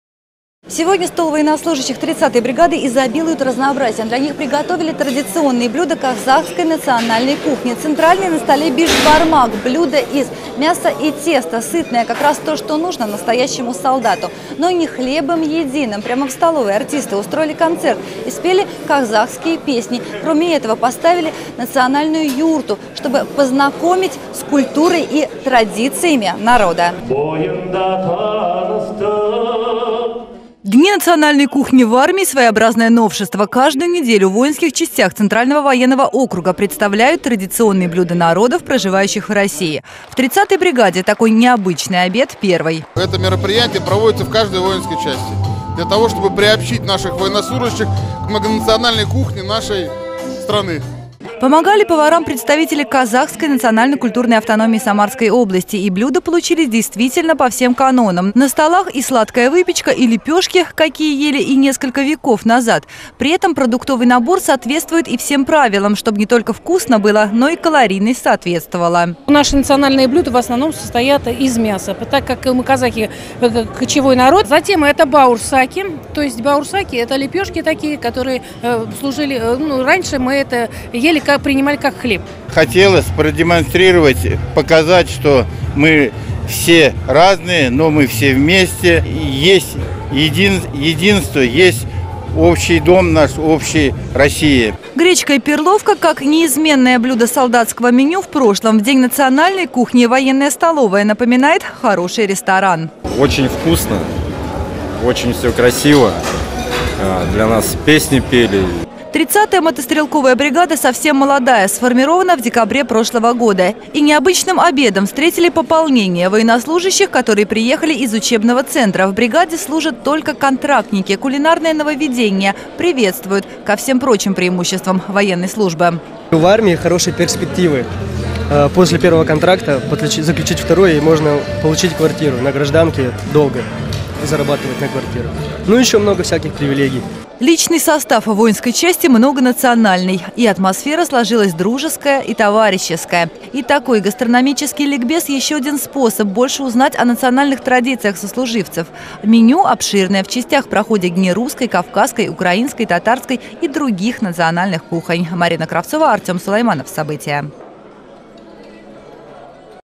Служить со вкусом. В 30-й мотострелковой бригаде прошел день национальной кухни
Служба со вкусом. Солдатское меню в воинских частях Центрального военного округа разнообразили национальным колоритом. Еженедельно военным подают блюда традиционной кухни народов  России. Разнообразие не только на столах, обед сопровождают культурной программой.